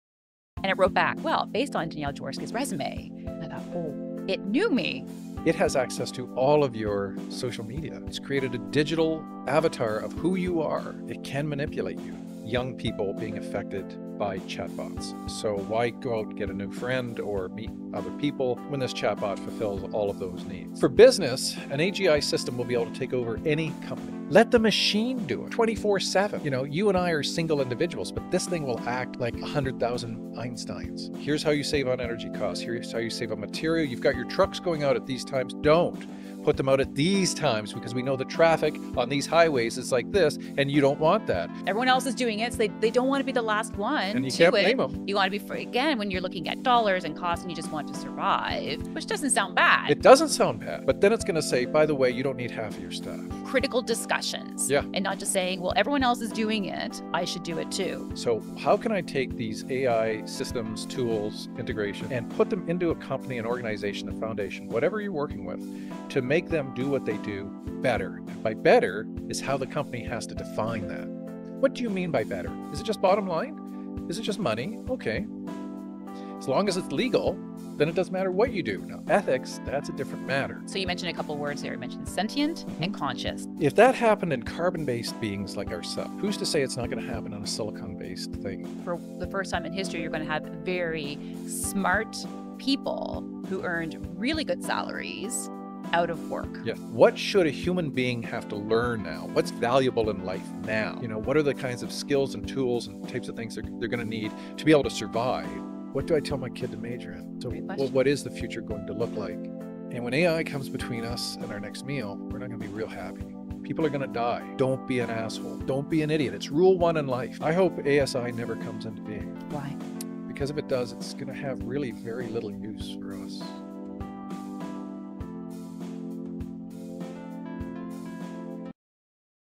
When AI Comes Between Us and Our Next Meal - Coming Soon
Can AI become conscious? Will AGI replace your job? Danielle Jaworski and Dr. Christopher DiCarlo (Ph.D. Critical Thinking & Ethics, Harvard postdoc) explore existential questions nobody wants to ask. From chatbots manipulating behavior to workforce automation, they discuss AI ethics and survival skills. Coming soon to Leaders' Mindset.

🌟 Your Leadership Journey Starts Here. Don’t just follow—lead.